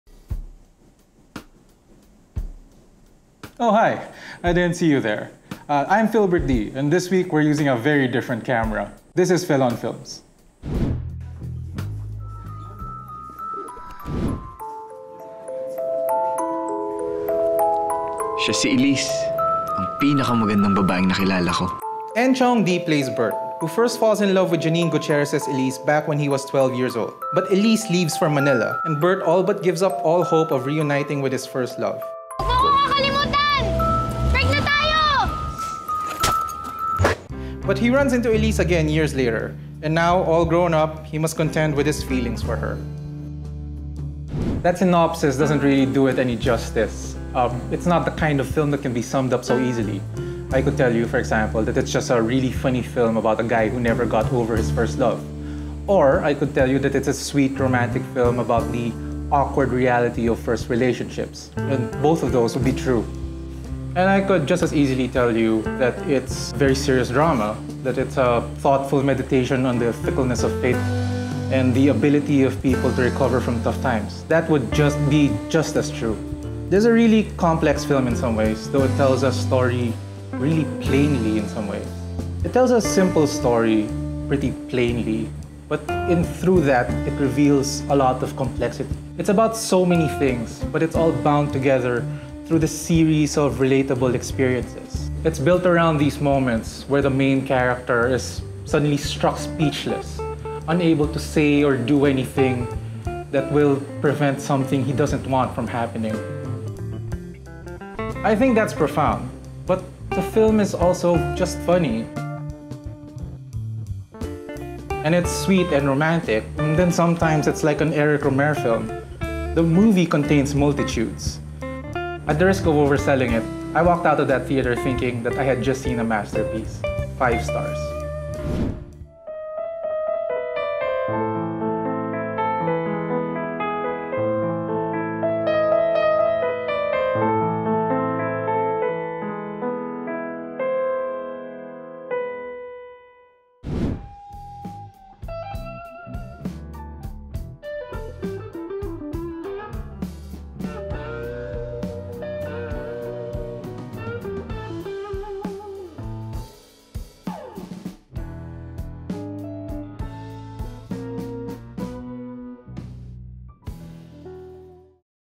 Elise Review
The original video that this review was part of was removed due to restrictive copyright policies. But here is the review of Elise, back online, just in time for its arrival on Netflix.